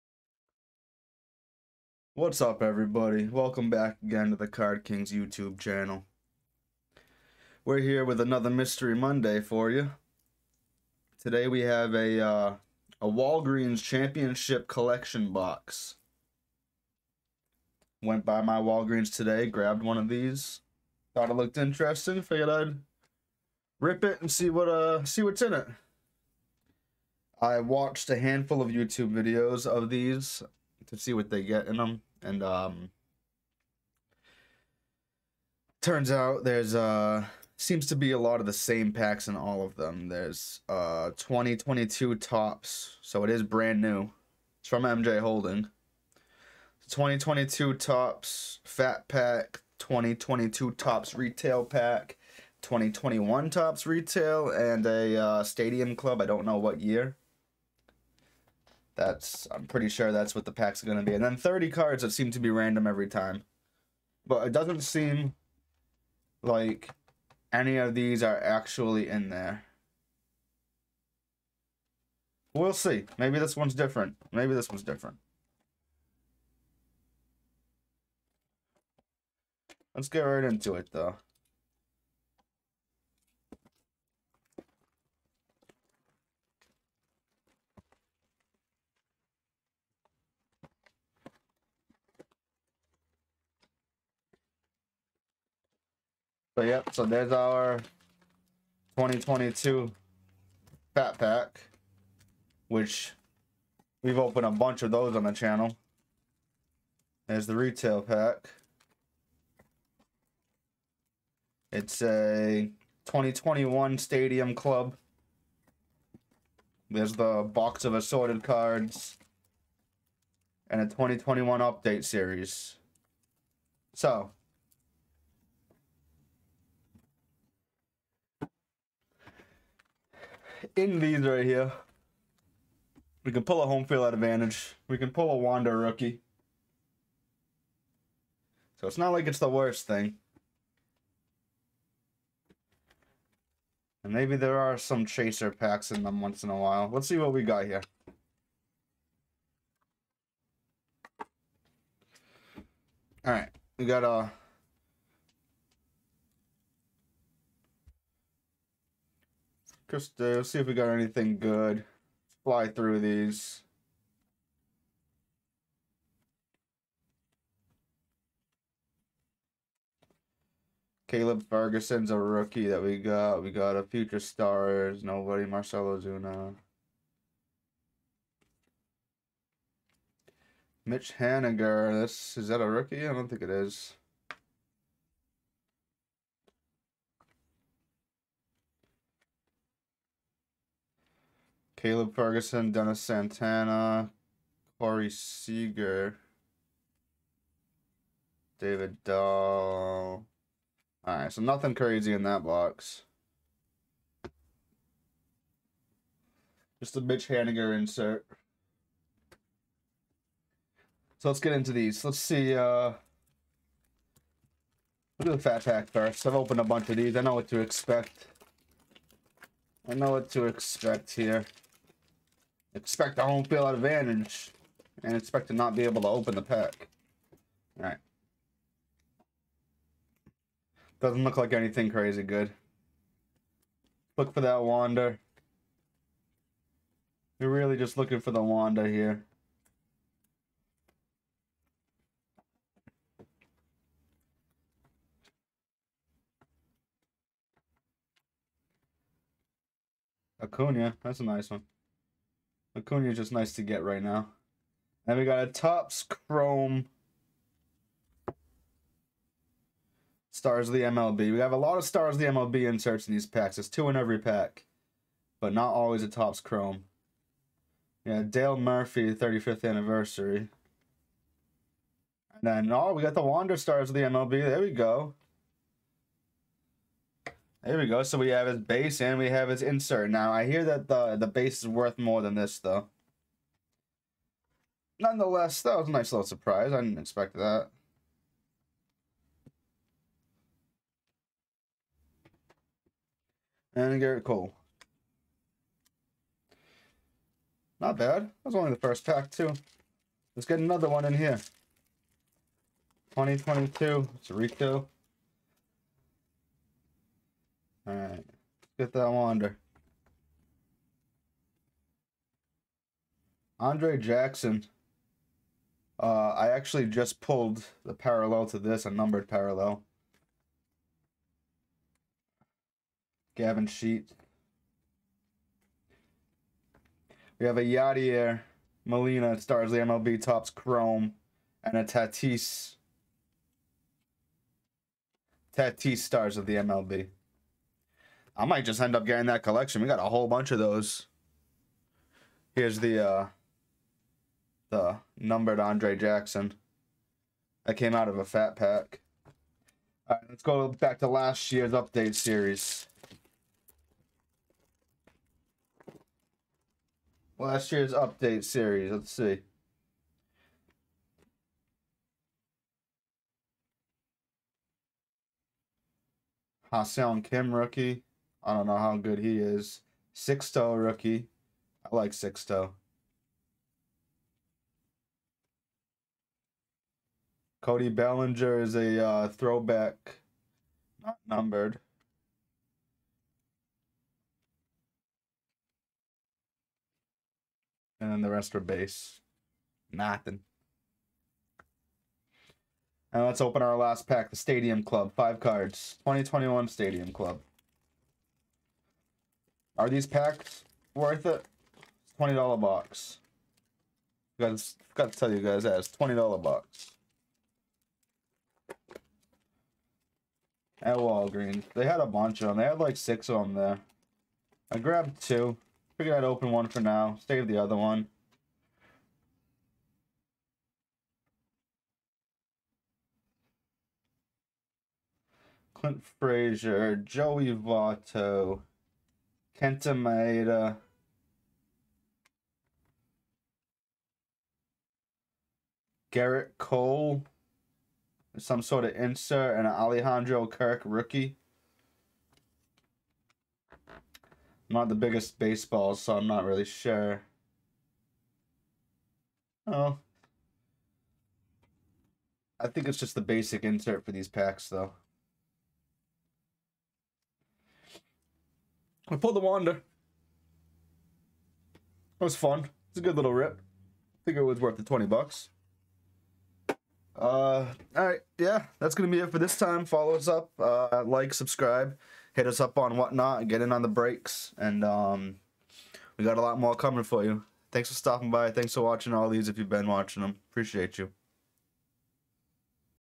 Mystery Monday 008: 2022 CHAMPIONSHIP CONTENDERS | WALGREENS MYSTERY BOX
This weeks Mystery Monday (on a Tuesday) opening a 2022 Championship Contenders Mystery Box from Walgreens